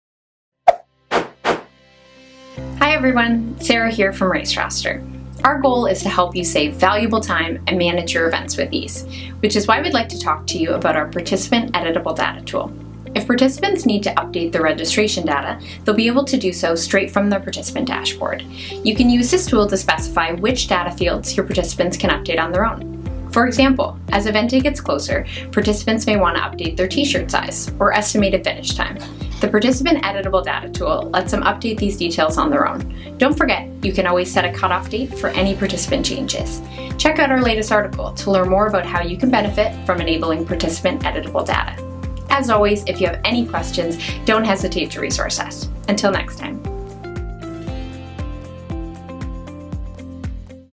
Provide enhanced support with the participant editable data tool
Providing quality customer service is essential to ensuring your participants reaching out for help feel supported. If data entered during registration needs to be updated, providing a fast and effective solution assures your participants their needs are important to you. With the participant editable data feature, you can specify which data fields your participants can update on their own, allowing you to efficiently handle multiple participant requests, freeing up more of your time to tackle other important tasks.